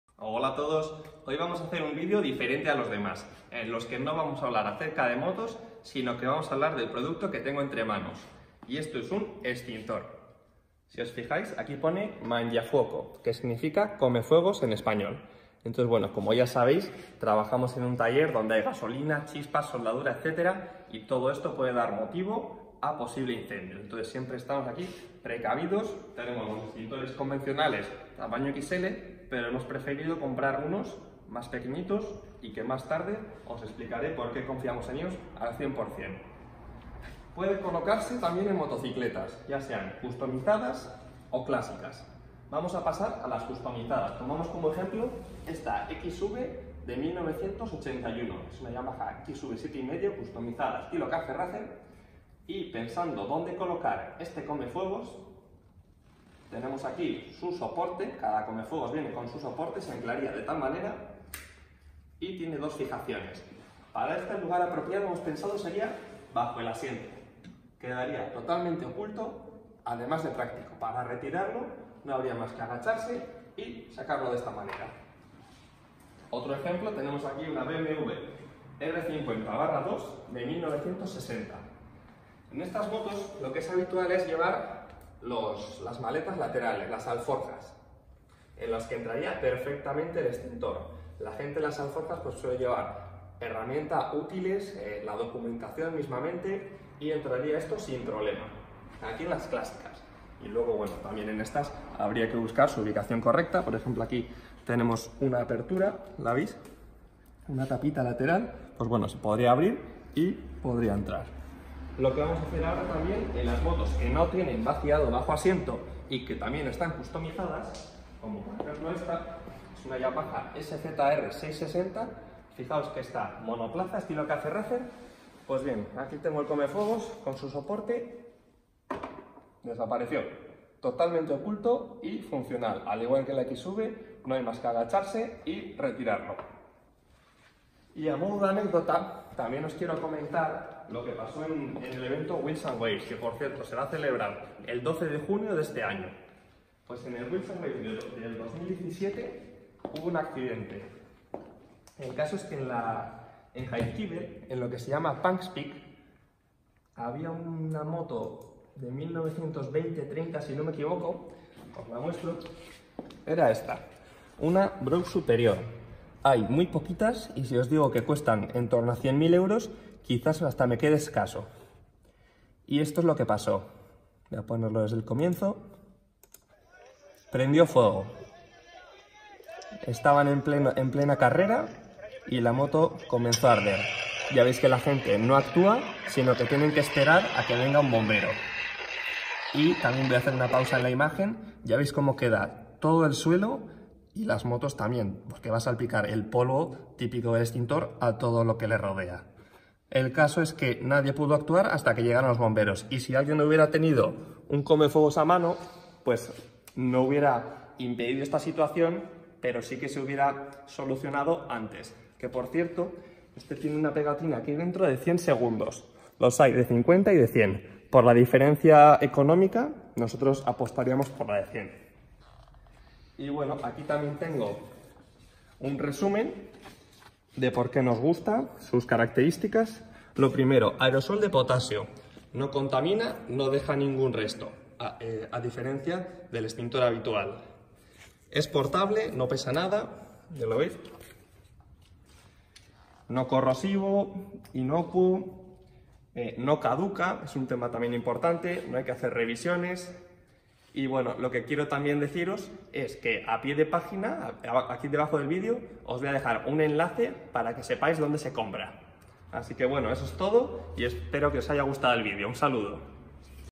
Algo que NO puede faltar en tú vehículo - Recomendación Cafe Racer SSpirit
Mostramos el extintor comefuegos en Cafe Racer SSpirit.